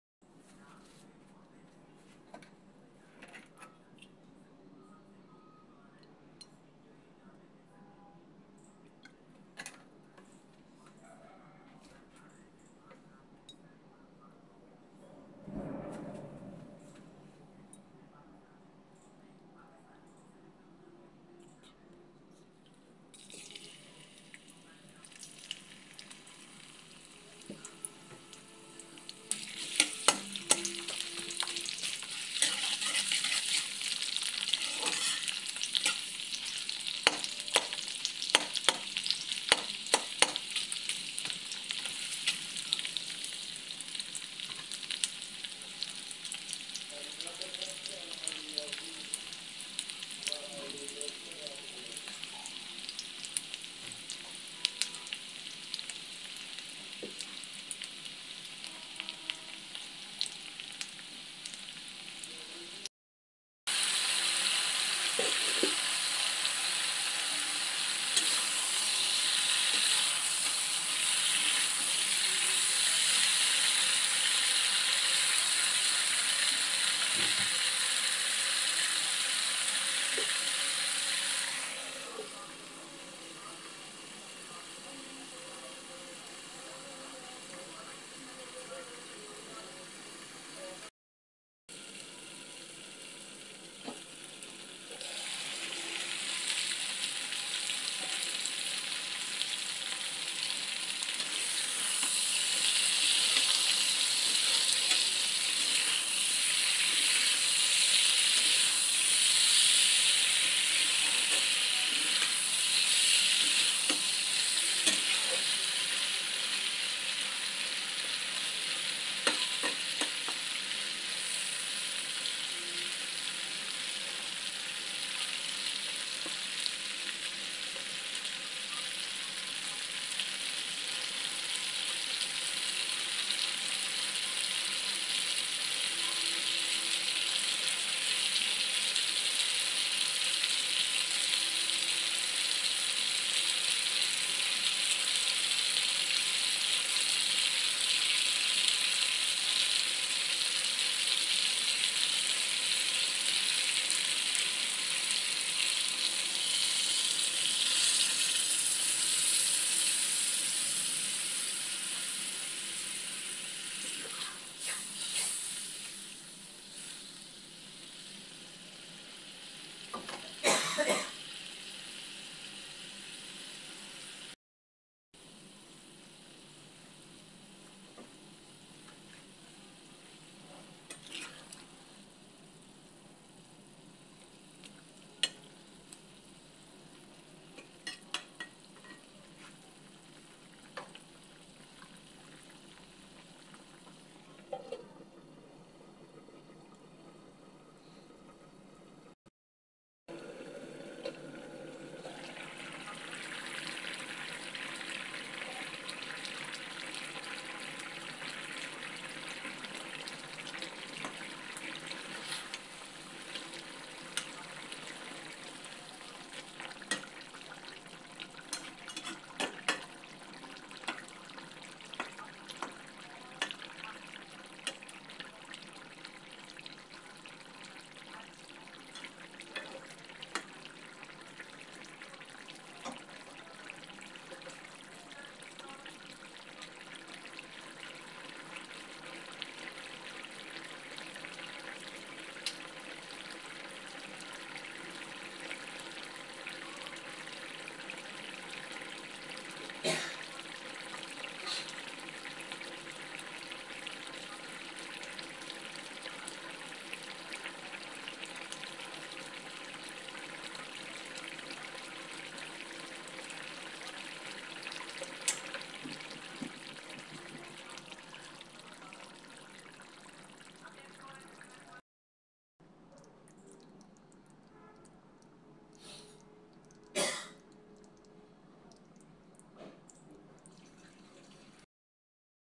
Savor the Flavors Potato Eggplant Soup Recipe for a Hearty Delight!
🥔🍆 Dive into the world of deliciousness with our Potato Eggplant Soup recipe! This hearty and wholesome soup is a perfect blend of comforting potatoes and the rich, velvety texture of eggplants. Join us in this culinary journey as we guide you through the step-by-step process of creating a bowlful of warmth and flavor. Whether you're a seasoned chef or a kitchen novice, this recipe is sure to satisfy your taste buds. Watch till the end for our pro tips and variations to make this soup uniquely yours.